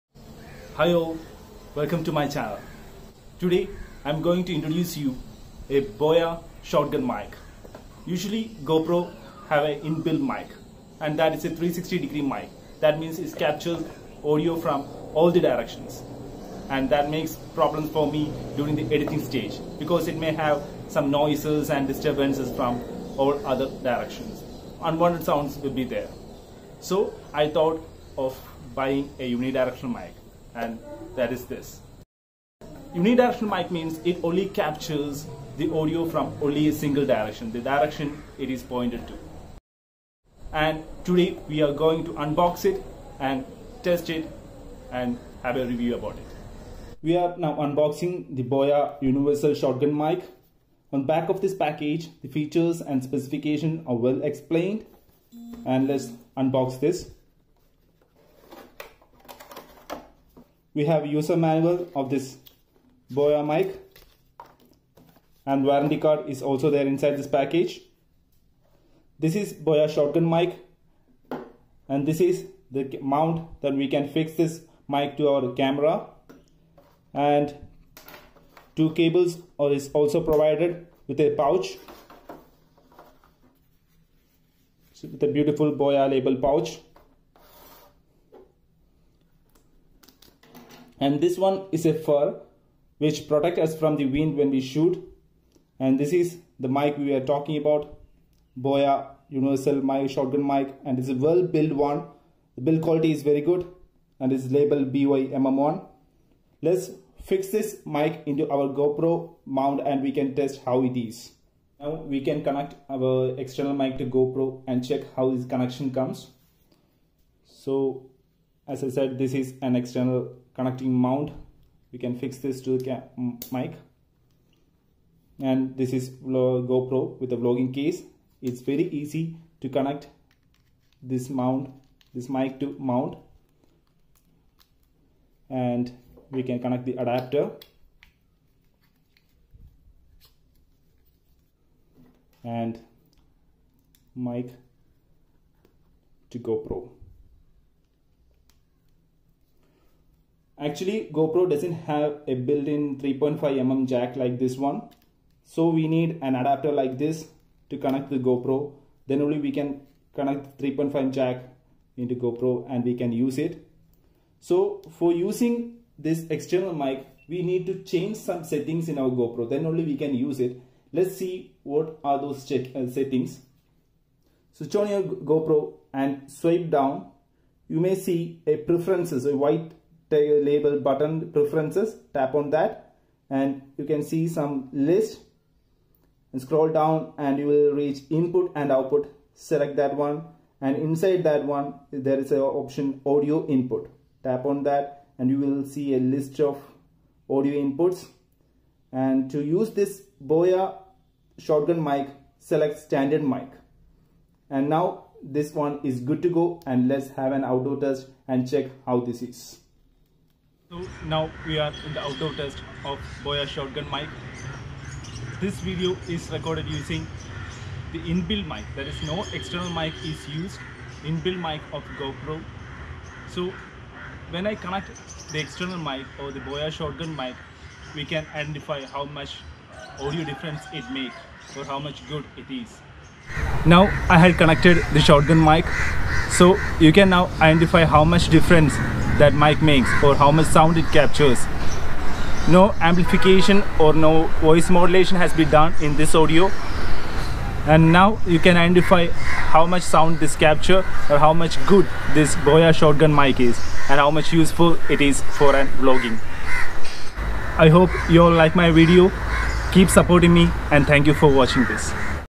Boya BY-MM1 Unboxing and Outdoor Testing in Gopro | Best Budget Mic For Vloggers | Gopro Settings
The product is definitely 5-star.
It's the cheapest cardiod-shotgun mic you can find.
Sound Clarity is awesome, even if their is a TV running behind and you speak on the mic
It can be used on Smartphones, cameras, camcorders, audio recorders, PCs, and other audio/video recording devices.

Fits for Smartphone, DSLR Camera Camcorder and Recorder. With 2 Cable, One is TRRS; one is TRS Cable.

No battery Required Do not need the battery, Just use it directly with your  smartphones or DSLR cameras.

Compact and Light Weight Only 86g/0.19lb, With a Portable Leather Case, you can take it anywhere.

Frequency Response：35-18KHz +/-3dB; Sensitivity：-42dB +/- 1dB / 0dB=1V/Pa, 1kHz; Signal to Noise Ratio: 76dB SPL

Rugged metal construction and anti-shock mount, which can effectively reduce unwanted vibration, cable and handling noise. Also included a furry windproof shield.